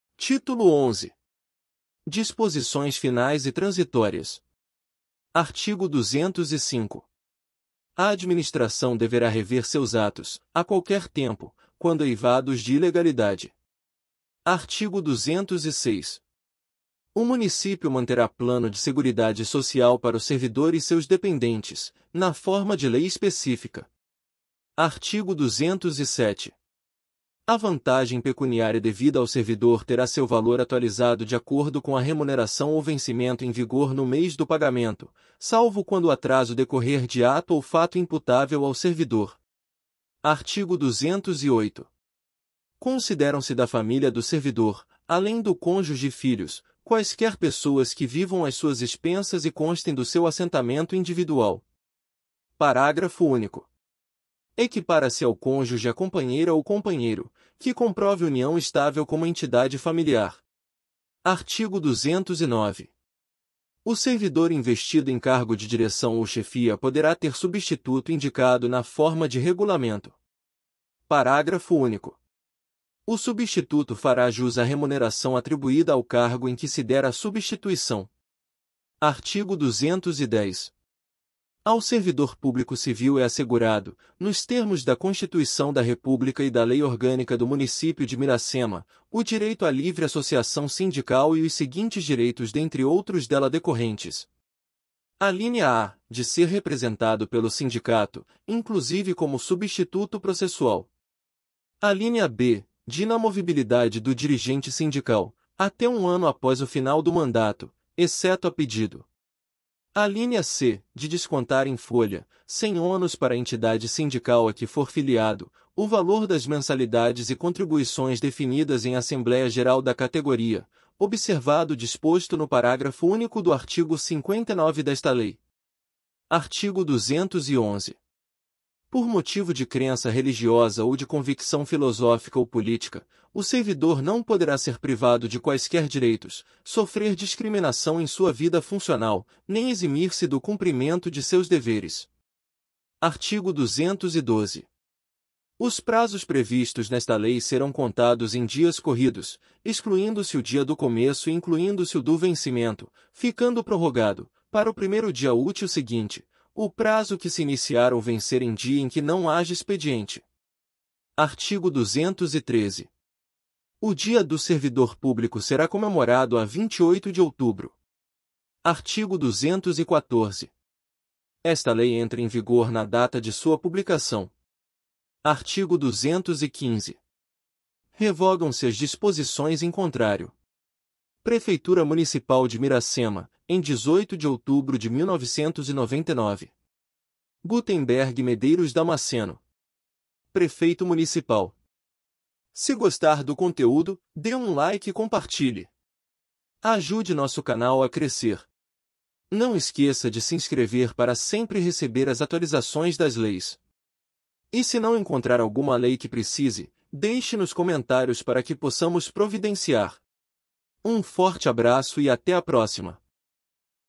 LEI Nº. 796, DE 18 DE OUTUBRO DE 1999 - ESTATUTO DOS SERVIDORES - TÍTULO XI
LEI COMPLEMENTAR Número 796, DE 18 DE OUTUBRO DE 1999
Altera disposições do Regime Jurídico dos Servidores Públicos Civis do Município de Miracema.

TÍTULO XI
DISPOSIÇÕES FINAIS E TRANSITÓRIAS